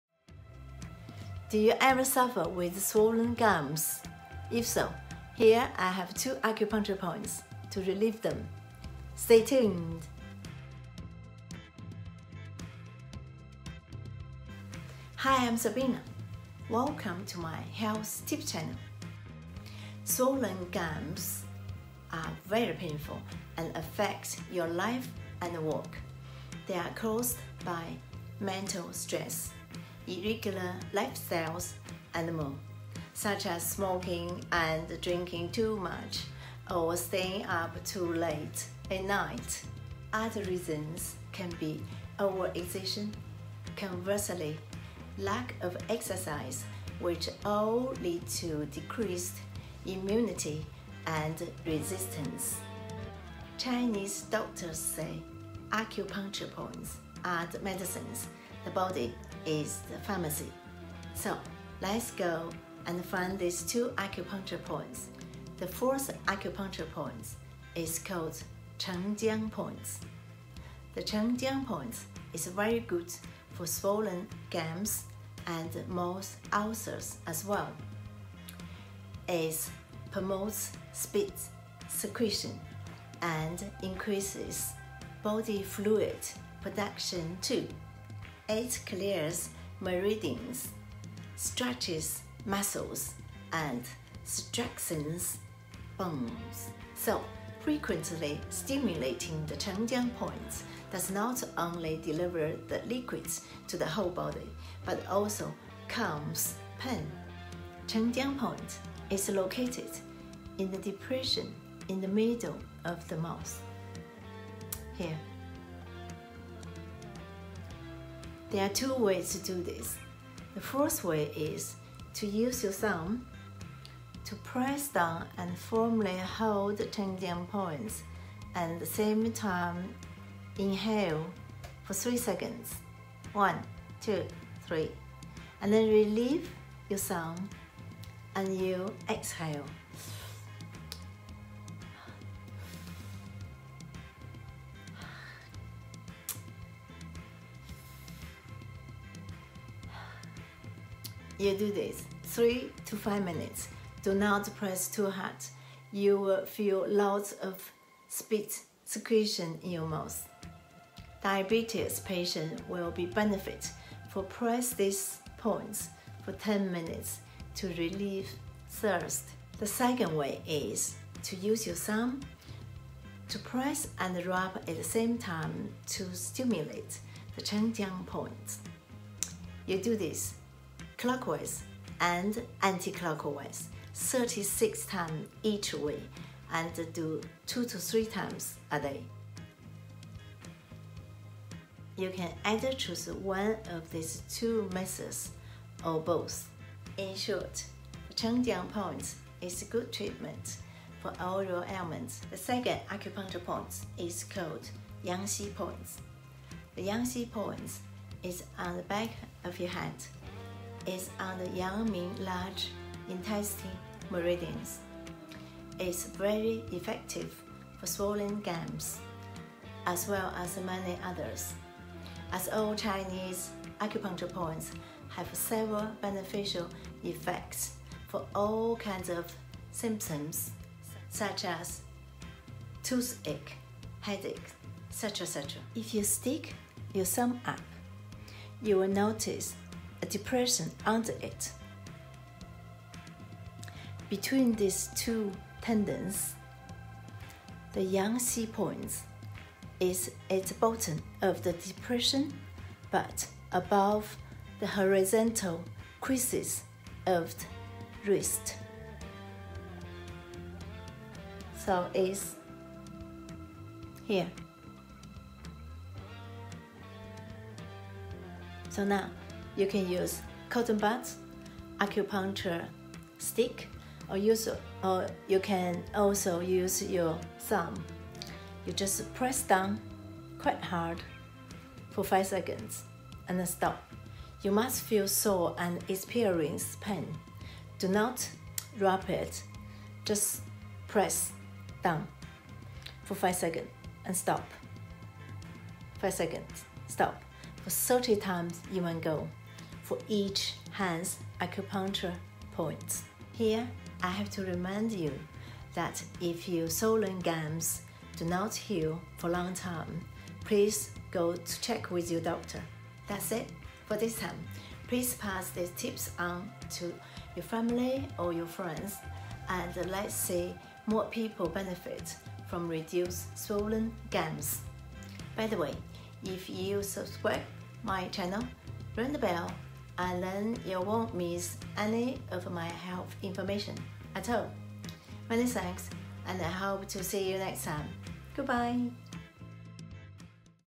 Relieve swollen gums with acupuncture points. Youtube 2021
Hi I am Serpina, welcome to my Health Tips Channel.

Chinese Doctors say: Acupuncture points are the medicines, the body is the pharmacy.

Here I have 2 acupuncture points to relieve swollen gums; follow my video and I am sure that you won’t regret it!

Please pass on these tips to your family and friends, and let’s see more people benefit from reduced pain!

Health Tips with Serpina